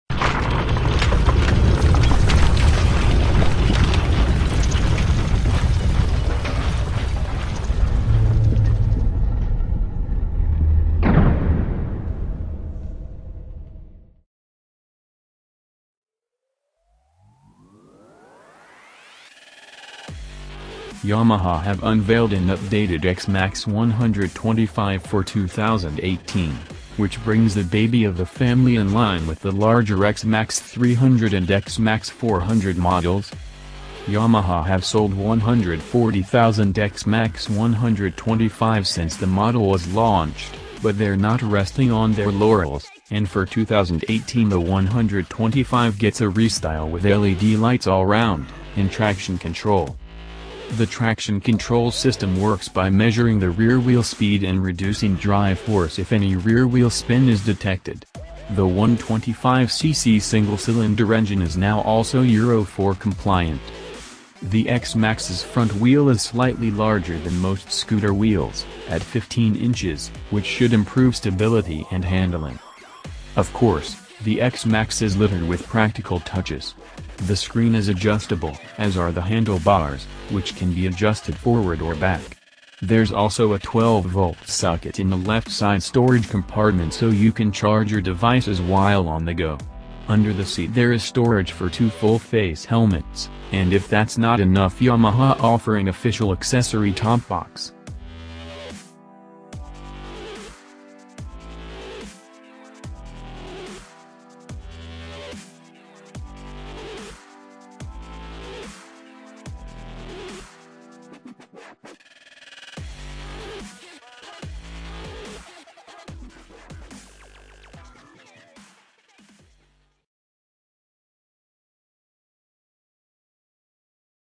WOW AMAZING!!2018 Yamaha X MAX 125 Price & Spec
WOW AMAZING!!2018 Yamaha X MAX 125 Price & Spec - Yamaha have unveiled an updated X-MAX 125 for 2018, which brings the baby of the family in line with the larger X-MAX 300 and X-MAX 400 models.

██████ GET DAILY UPDATES! NOW! ██████